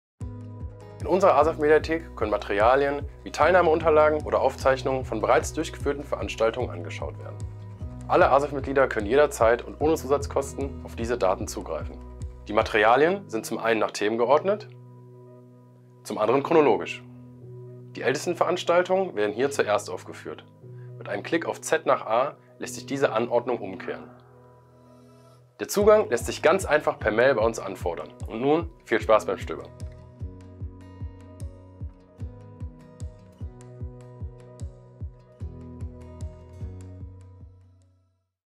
Die neue ASEW-Mediathek
ASEW-Mitglieder haben Zugriff auf Veranstaltungsunterlagen und Aufzeichnungen vergangener Veranstaltungen.